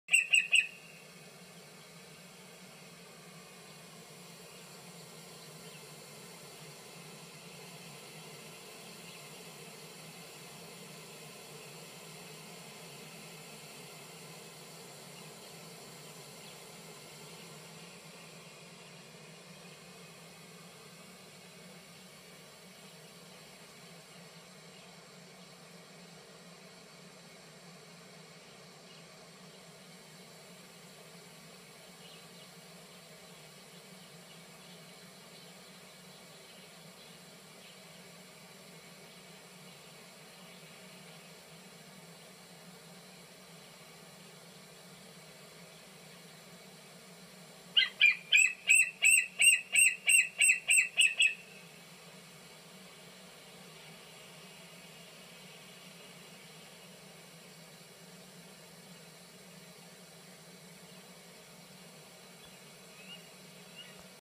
Catalina Island sits 22 miles off the coast of southern California
Catalina Island sits 22 miles off the coast of southern California, and its rocky cliffs are the perfect nesting location for bald eagles.. young baby eagle waiting for food.